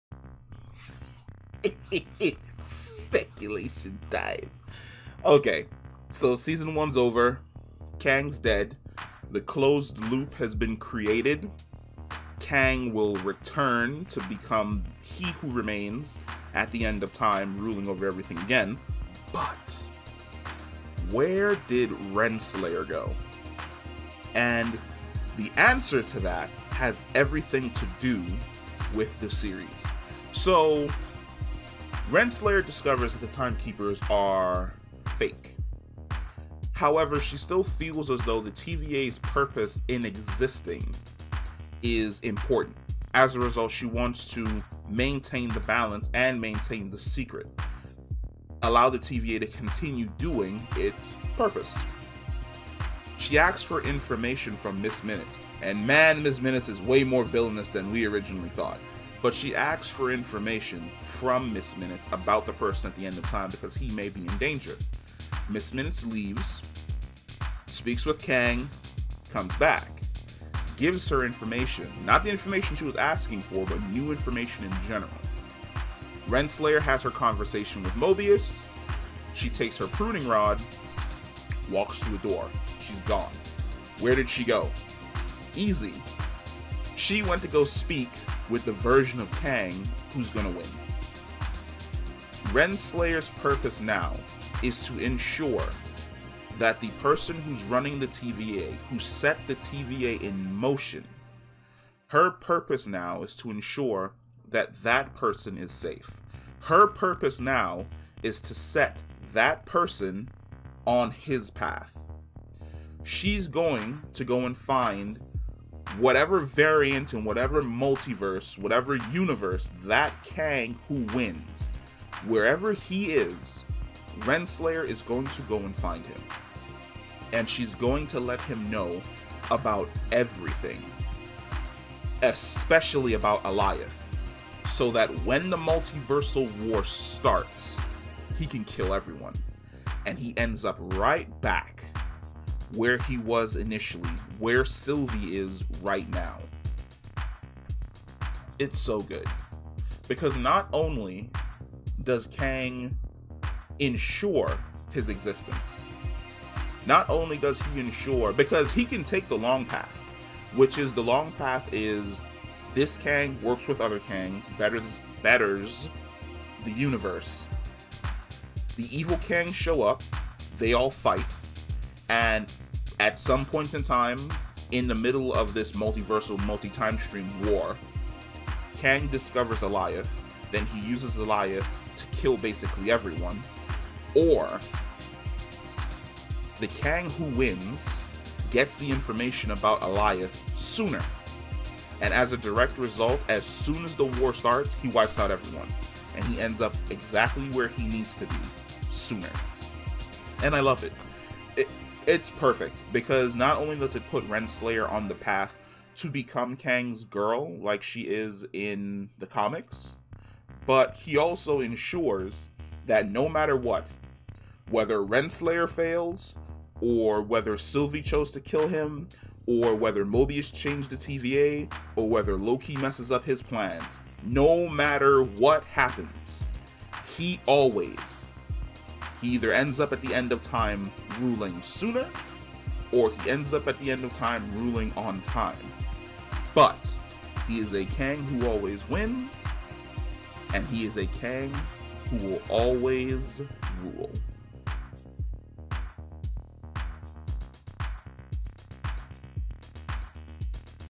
Where Did Renslayer Go? | Off To Save The TVA? Sacred Timeline Or Kang? | Loki Season Finale
At the end of Loki episode 6 Renslayer went through a time door after speaking with Mobius to parts unknown. Where did she go? Who has she gone to see and What information did Miss Minutes give her?